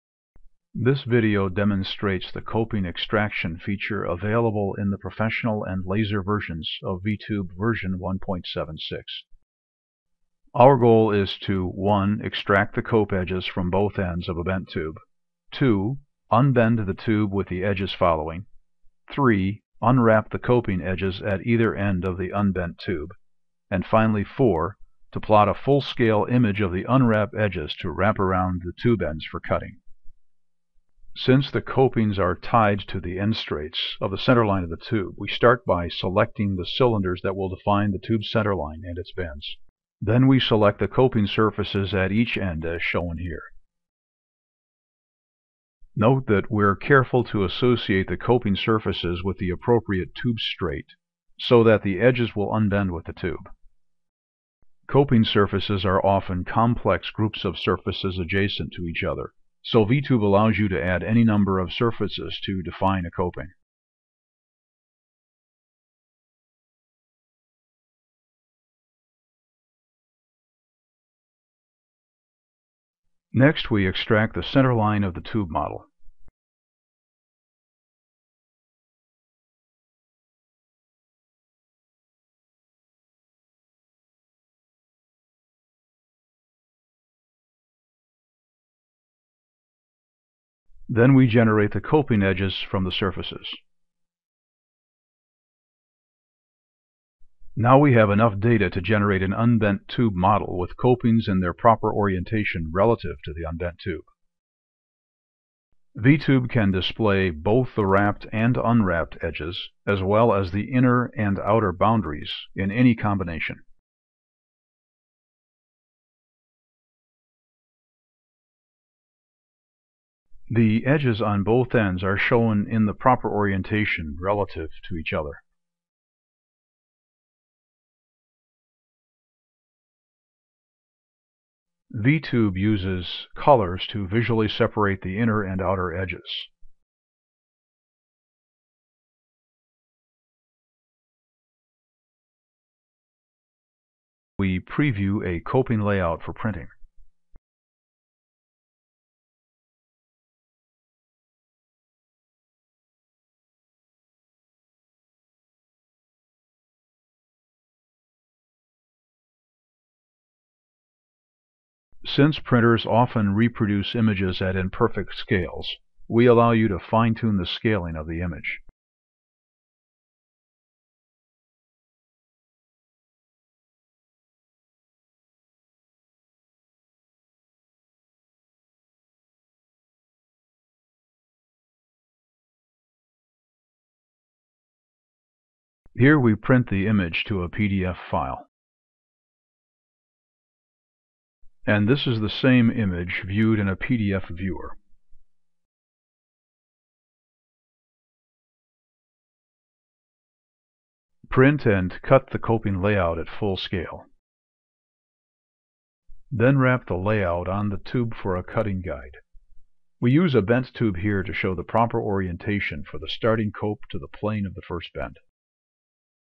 Unbending a Tube with Coping at Either End using VTube
Use VTube-PRO or VTube-LASER to unbend a tube with the copings following the unbending. Then print coping layouts to wrap around the tube for cutting the tube before bending.